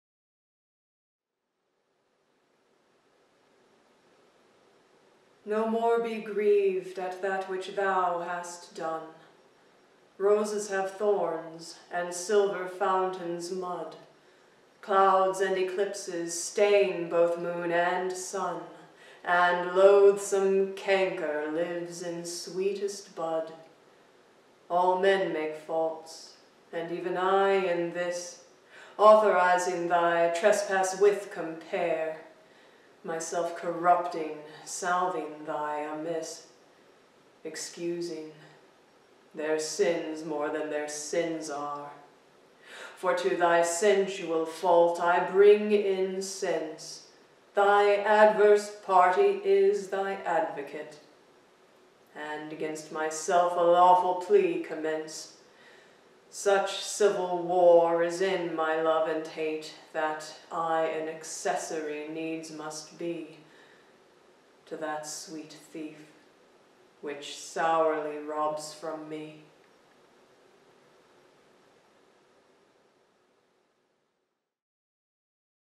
Shakespeare's Sonnet 35: Fall 2017
Christine Williamson of Moonsong Studios performs William Shakespeare's Sonnet 35.